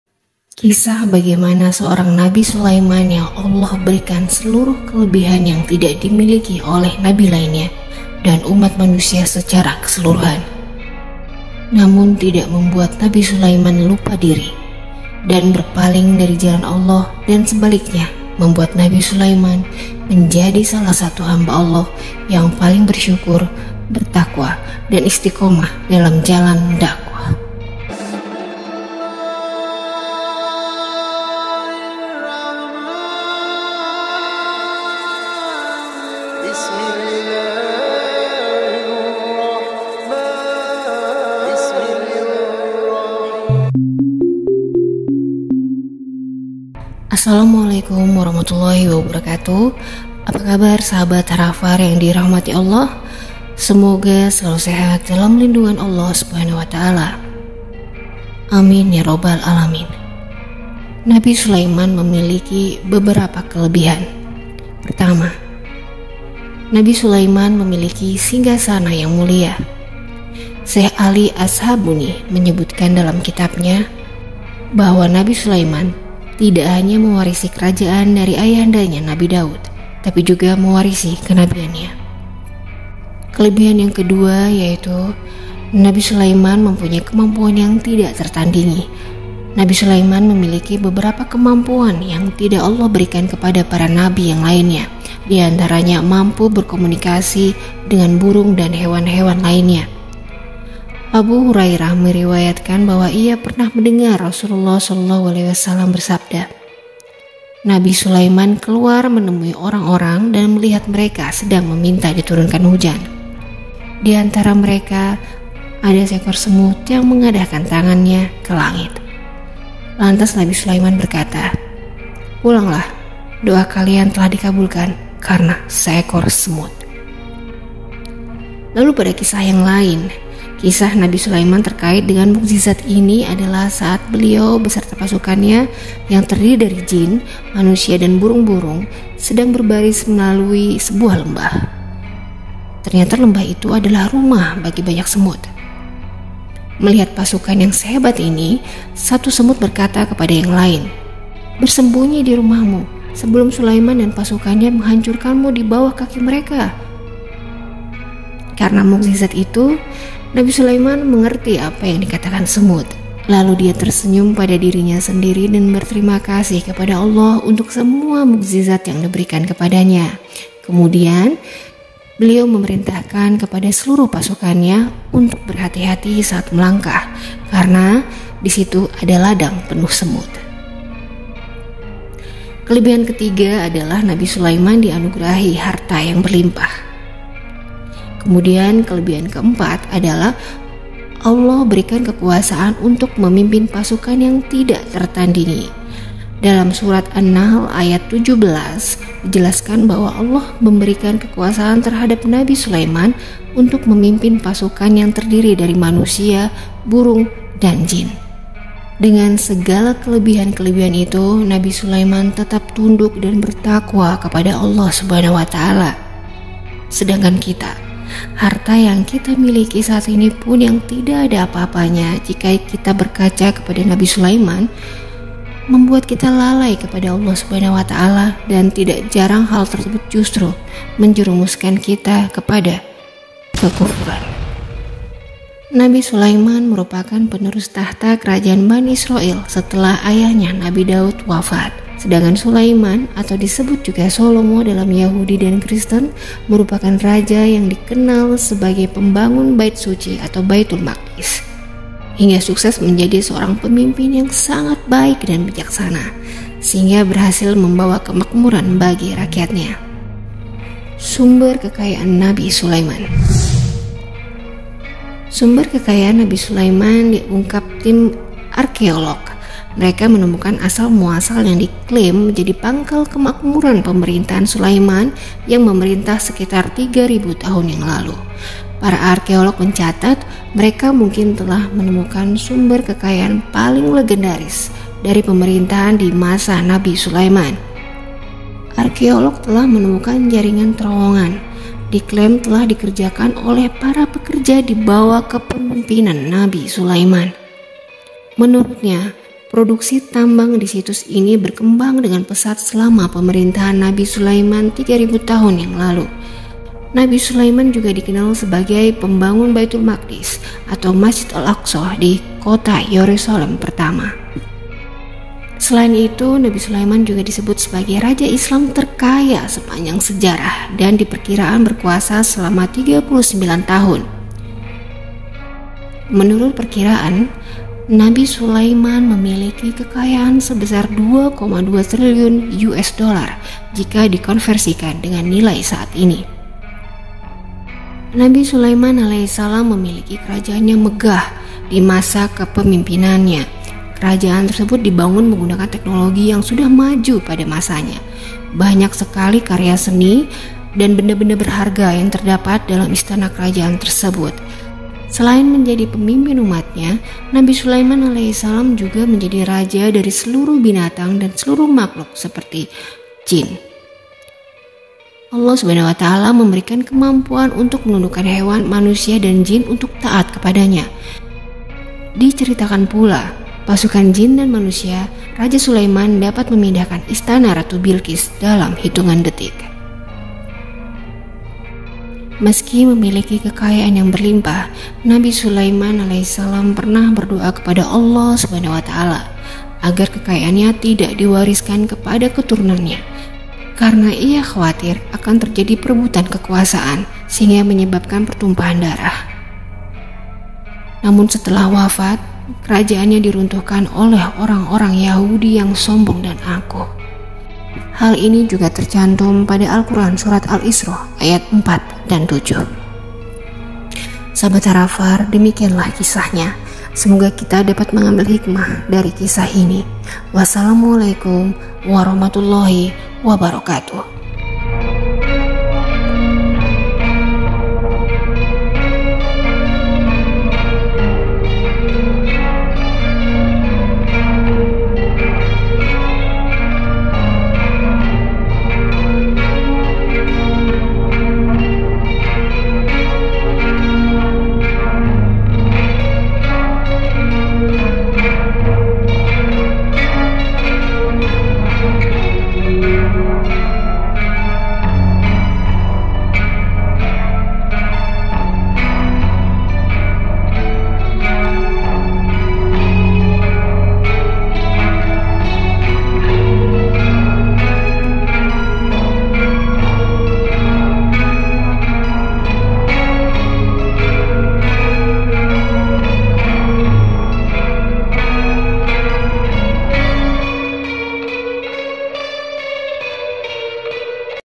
kisah Nabi Sulaiman | Rahasia Kekayaan nabi sulaiman
kisah bagaimana seorang Nabi Sulaiman yang Allah berikan seluruh kelebihan yang tidak dimiliki Nabi lainnya dan ummat manusia secara keseluruhan, namun tidak membuat Nabi Sulaiman lupa diri dan berpaling dari jalan Allah dan sebaliknya membuat Nabi Sulaiman menjadi salah satu hamba yang paling bersyukur, bertaqwa dan isitqomah dalam jalan dakwah.
Kelebihan nabi sulaiman.
Pertama
Nabi Sulaiman Alaihissalam memiliki singgasana yang Mulia. Syaikh Ali Ash Shabuni menyebutkan dalam kitabnya
menyebutkan bahwa Nabi Sulaiman tidak hanya mewarisi kerajaan dari Ayahandanya Nadi Daud, tapi juga mewarisi keNabian.
Kedua
kemampuan yang tidak tertandingi .Nabi Sulaiman memiliki beberapa kemampuan yang tidak Allah berikan kepada para Nabi yang lain diantaranya mampu berkomunikasi (memehami bahasa) dengan burung dan hewan hewan lainnya.
Abu Hurairah meriwayatkan bahwa ia pernah mendengar Rasulullah SAW bersabda, "Nabi Sulaiman keluar menemui orang-orang dan melihat mereka sedang meminta diturunkan hujan. Di antara mereka ada seekor semut yang menengadahkan tangannya ke langit. Lantas Nabi Sulaiman berkata, 'Pulanglah! Doa kalian telah dikabulkan karena seekor semut'."
Lalu pada kisah yang lain, kisah Nabi Sulaiman terkait dengan mukjizatnya ini adalah saat beliau beserta pasukannya yang terdiri dari jin, manusia dan burung-burung, sedang berbaris melalui sebuah lembah. Ternyata, lembah itu adalah rumah bagi banyak semut.
Melihat pasukan yang hebat ini, satu semut berkata kepada yang lain: "Bersembunyi di rumahmu, sebelum Sulaiman dan pasukannya menghancurkanmu di bawah kaki mereka!". Karena mukjizatnya itu, Nabi Sulaiman mengerti apa yang dikatakan semut.
Dia tersenyum pada dirinya sendiri dan berterima kasih kepada Allah untuk semua mukjizat yang diberikan kepadanya. Kemudian, dia memerintahkan kepada seluruh pasukannya untuk berhati-hati saat melangkah, karena itu adalah ladang penuh semut.
Ketiga
Ketiga, Nabi Sulaiman dianugerahi Harta yang Berlimpah.
Keempat,
Allah berikan kekuasaan untuk memimpin pasukan yang tidak tertandingi.Dalam  surat An-Nahl ayat 17 dijelaskan bahwa Allah berikan kekuasaan terhadap Nabi Sulaiman untuk memimpin pasukan yang terdiri dari Manusia, Burung & Jin.
Dengan segala kelebihan kelebihan itu Nabi Sulaiman  tetap tunduk dan bertaqwa kepada Allah Subhanahu Wata’ala. Sedangkan kita
harta yang kita miliki saat ini pun yang tidak apa-apanya jika kita berkaca kepada Nabi Sulaiman, membuat kita lalai kepada Allah Subhanahu wa ta’ala dan tidak jarang hal tersebut justru menjerumuskan kita kepada kekufuran.
Nabi Sulaiman merupakan penerus tahta kerajaan Bani Israil setelah Nabi Daud (ayahnya) wafat.
Sedangkan Sulaiman atau disebut juga Solomo dalam Yahudi dan Kristen merupakan raja yang dikenal sebagai pembangun Bait Suci atau Baitul Maqdis.
nabi sulaiman diutus Allah SWT untuk memimpin Bani Israil. Hingga sukses menjadi seorang pemimpin yang sangat baik dan bijaksana. Sehingga berhasil membawa kemakmuran bagi rakyatnya.
Sumber kekayaan Nabi Sulaiman diungkap tim arkeolog. Mereka menemukan asal muasal yang diklaim menjadi pangkal kemakmuran pemerintahan Sulaiman yang memerintah sekitar 3.000 tahun yang lalu.
para arkeolog mencatat, mereka mungkin telah menemukan sumber kekayaan paling legendaris dari pemerintahan di masa Nabi Sulaiman.
arkeolog telah menemukan jaringan terowongan. Diklaim telah dikerjakan oleh para pekerja di bawah kepempimpinan Nabi Sulaiman.
Menurutnya, produksi tambang di situs itu berkembang dengan pesat, selama pemerintahan Nabi Sulaiman 3.000 tahun yang lalu.
Nabi Sulaiman juga dikenal sebagai pembangun Baitul Maqdis atau Masjid Al-Aqsa di kota Yerusalem pertama.

Selain itu, nabi Sulaiman juga disebut sebagai raja Islam terkaya sepanjang sejarah dan diperkirakan berkuasa selama 39 tahun. Menurut perkiraan, nabi Sulaiaman memiliki kekayaan sebesar 2,2 triliun USD jika dikonversikan dengan nilai saat ini.